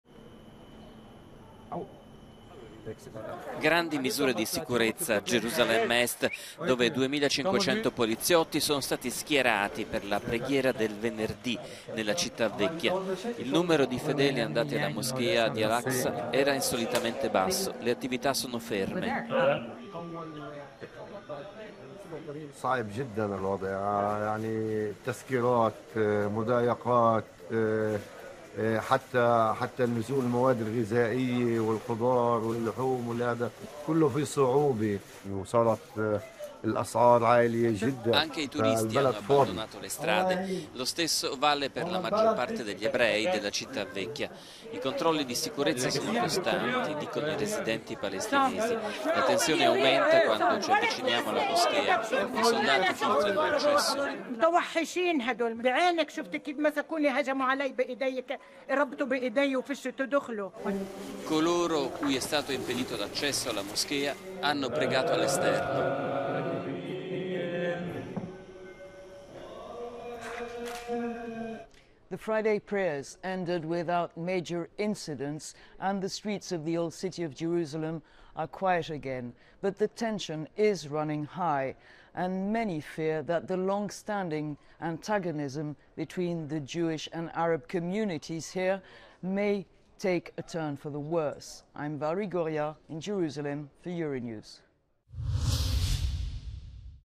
Gerusalemme Vecchia: tensione alle stelle alla moschea Al Aqsa, migliaia di soldati
Israele ha schierato migliaia di soldati nelle strade della città vecchia che portano alla moschea Al Aqsa. Molti accessi vietati ai palestinesi. Nemmeno un turista per le strade. I fedeli costretti a pregare fuori